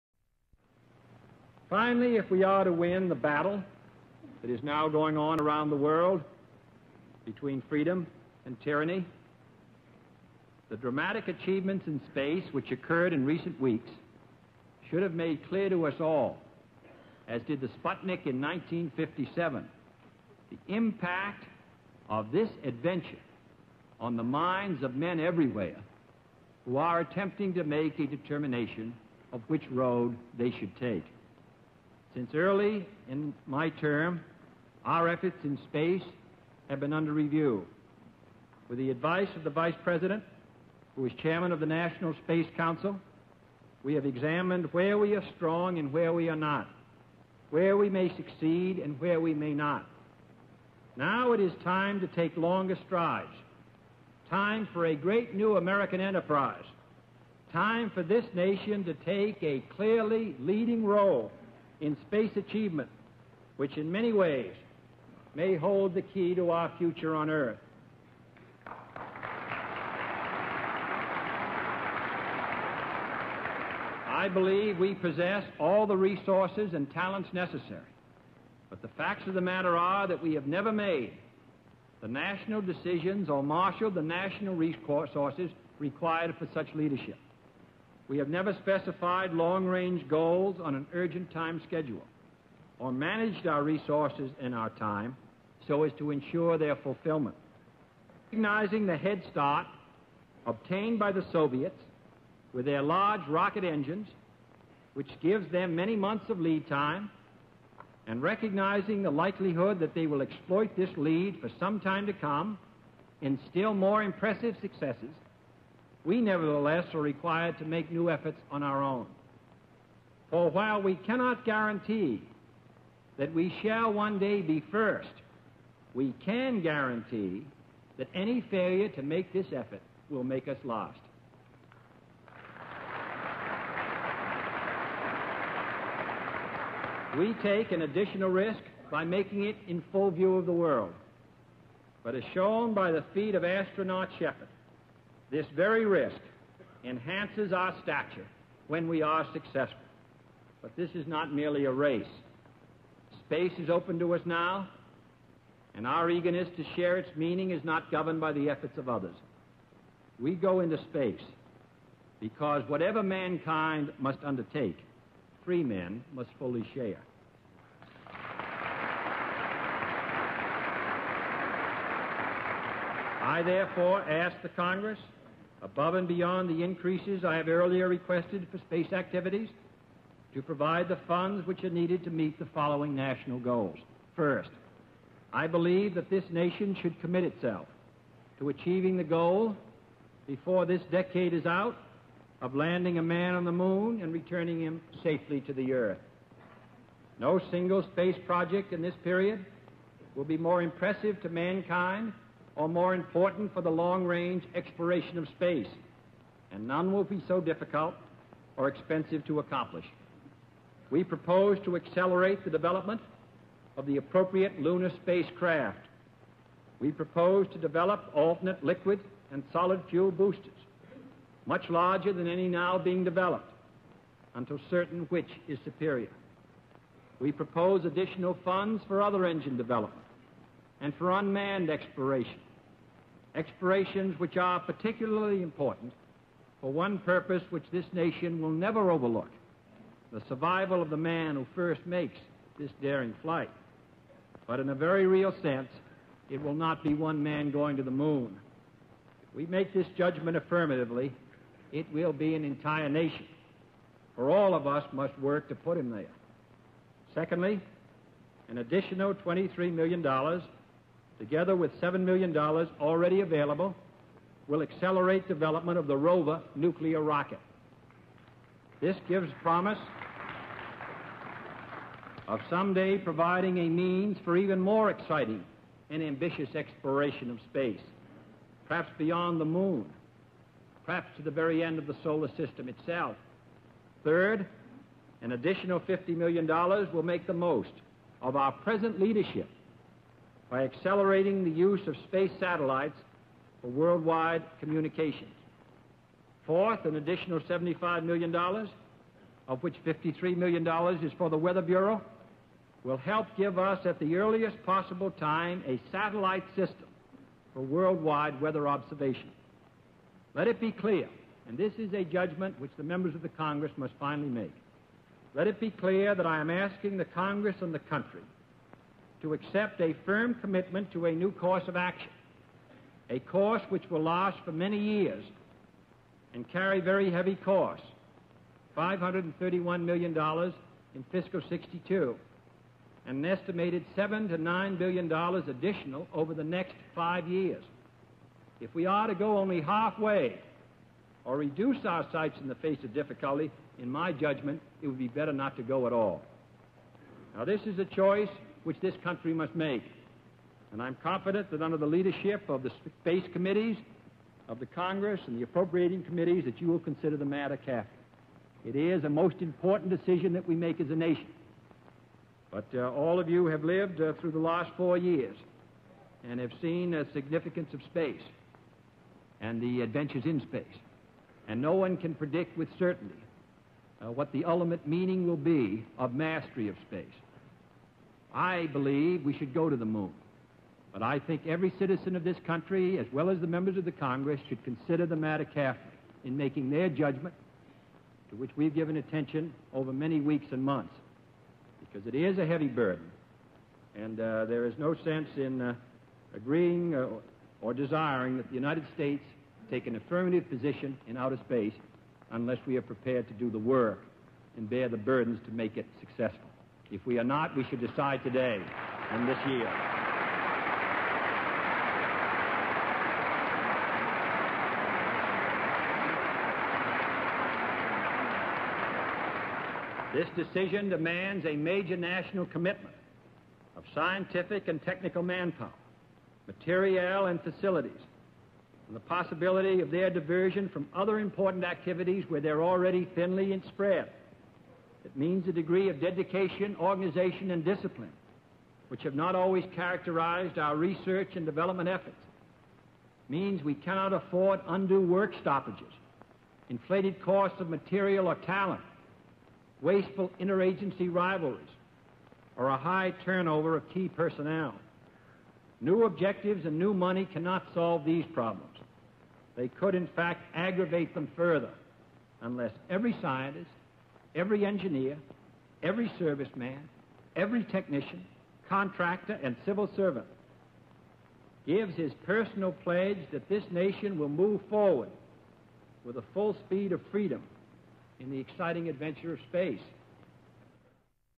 President John F. Kennedy, "On Urgent National Needs," Space Excerpt, May 25, 1961
In May 25, 1961, President John F. Kennedy delivered a speech to Congress titled, "On Urgent National Needs."  The theme of the speech was to demonstrate American strength in light of recent advances of Soviet interests.

This excerpt, in the speech text titled, "IX. SPACE," shows Kennedy's proposal to send the first astronauts to the Moon.

The complete text of the speech is on the JFK Library web

The video excerpt is on the JFK Library web